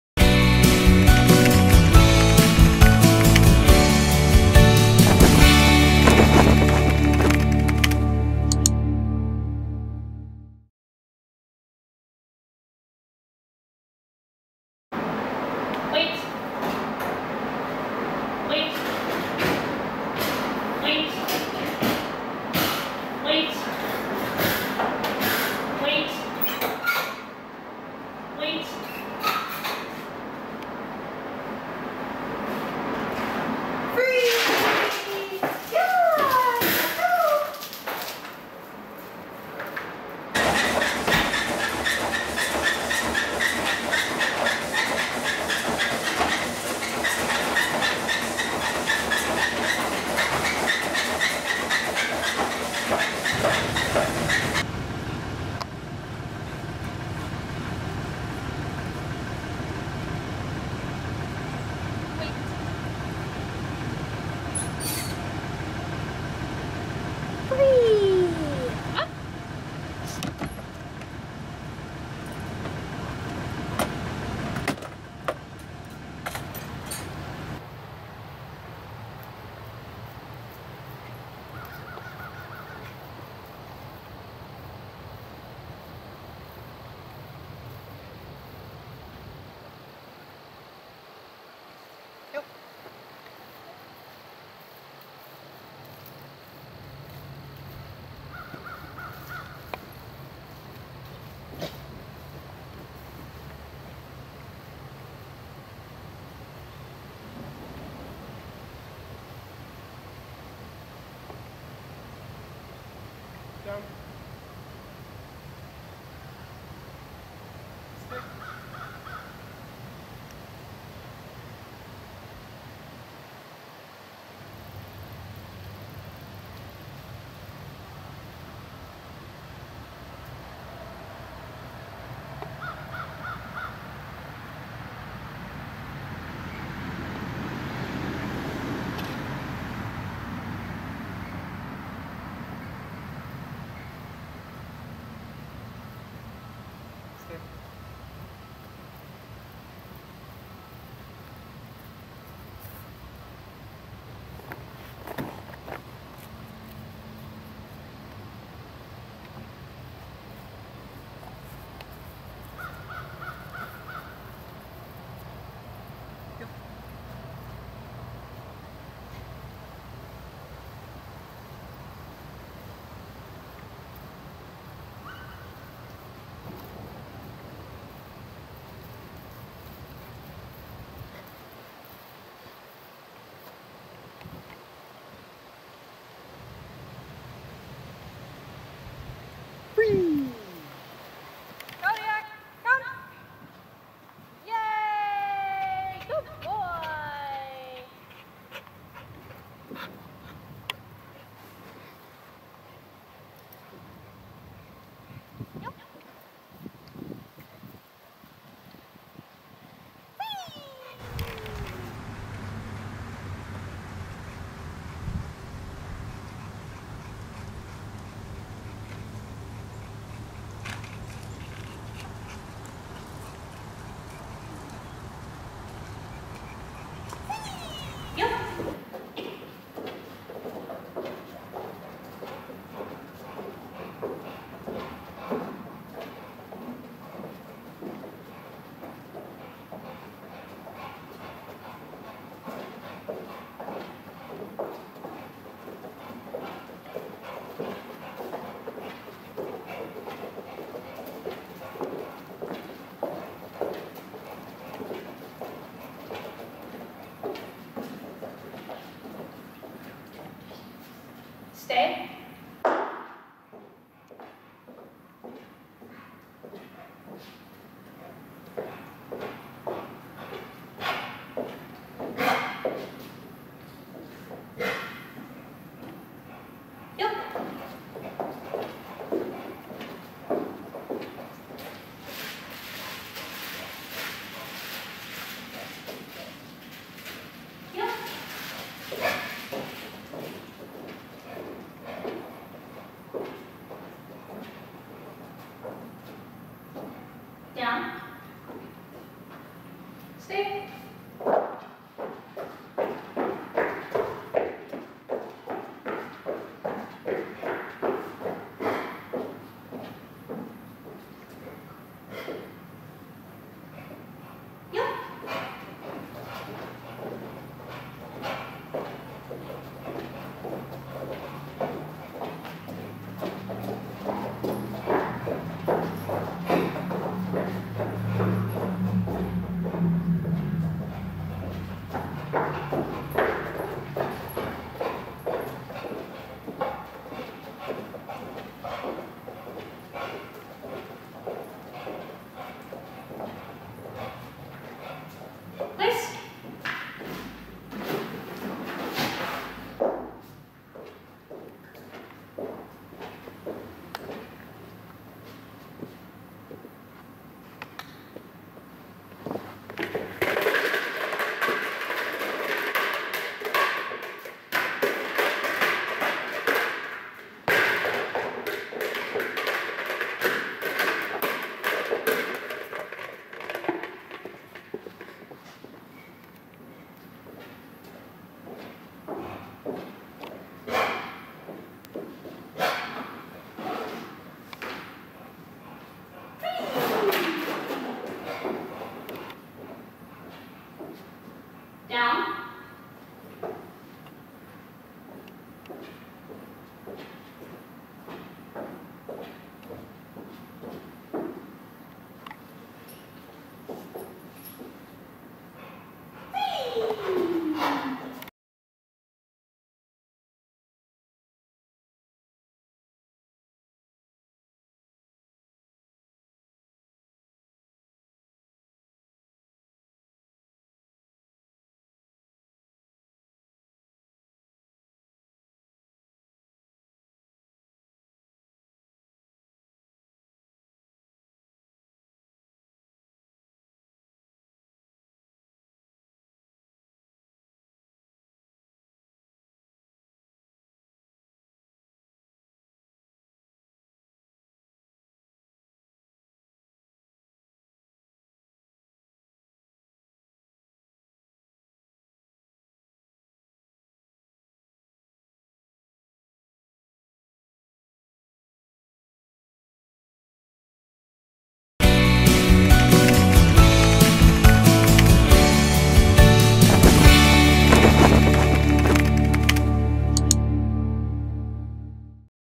Kodiak (Olde English Bulldog) Boot Camp Dog Training Video Demonstration
Kodiak (Olde English Bulldog) graduated from the dog training boot camp at Neuman K-9 Academy. This program included obedience commands to sit, stay, heel or walk on a loose leash, come when called, proper etiquette, no jumping up, meeting and greeting people under control, and running on a treadmill.

Our dog training camp provides programs for Olde English Bulldogs such as boot camp, obedience training, and puppy camp.